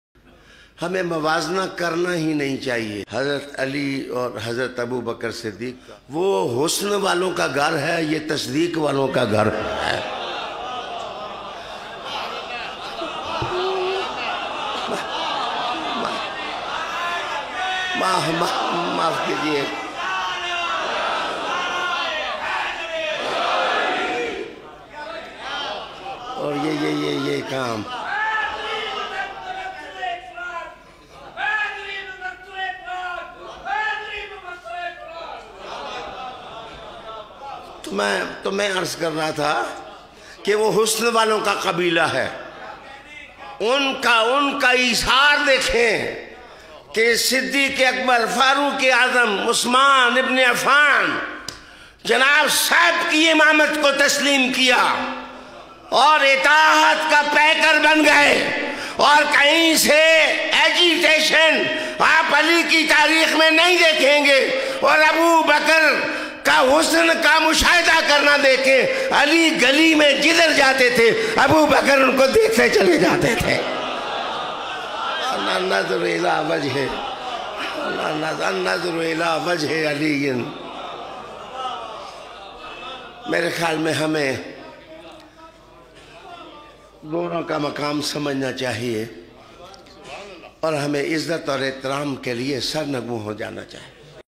Hazrat Ali A.S vs Hazrat Abu Bakkar A.S speech by Syed Riaz Hussain Shah sahab
humain mawazna krna he nhi Chaye